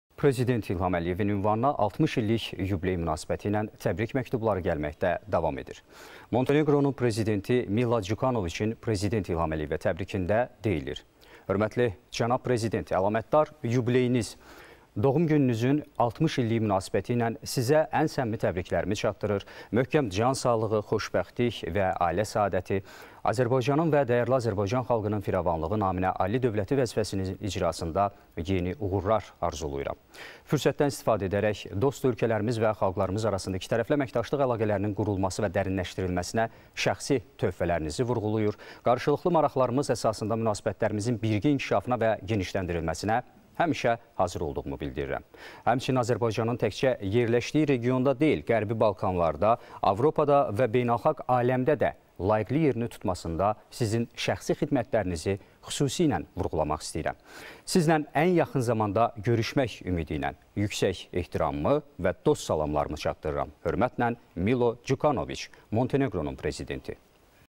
Monteneqro Prezidentinin təbriki
Azərbaycan Respublikasının Prezidenti Zati-aliləri cənab İlham Əliyevə

Hörmətli cənab Prezident.

Əlamətdar yubileyiniz – doğum gününüzün 60 illiyi münasibətilə Sizə ən səmimi təbriklərimi çatdırır, möhkəm cansağlığı, xoşbəxtlik və ailə səadəti, Azərbaycanın və dəyərli Azərbaycan xalqının firavanlığı naminə ali dövləti vəzifənizin icrasında yeni uğurlar arzulayıram.

Fürsətdən istifadə edərək, dost ölkələrimiz və xalqlarımız arasında ikitərəfli əməkdaşlıq əlaqələrinin qurulması və dərinləşdirilməsinə şəxsi töhfələrinizi vurğulayır, qarşılıqlı maraqlarımız əsasında münasibətlərimizin birgə inkişafına və genişləndirilməsinə həmişə hazır olduğumu bildirirəm.

Həmçinin Azərbaycanın təkcə yerləşdiyi regionda deyil, Qərbi Balkanlarda, Avropada və beynəlxalq aləmdə də layiqli yerini tutmasında Sizin şəxsi xidmətlərinizi xüsusilə vurğulamaq istəyirəm.

Sizinlə ən yaxın zamanda görüşmək ümidi ilə yüksək ehtiramımı və dost salamlarımı çatdırıram.

Hörmətlə,

Milo Cukanoviç

Monteneqronun Prezidenti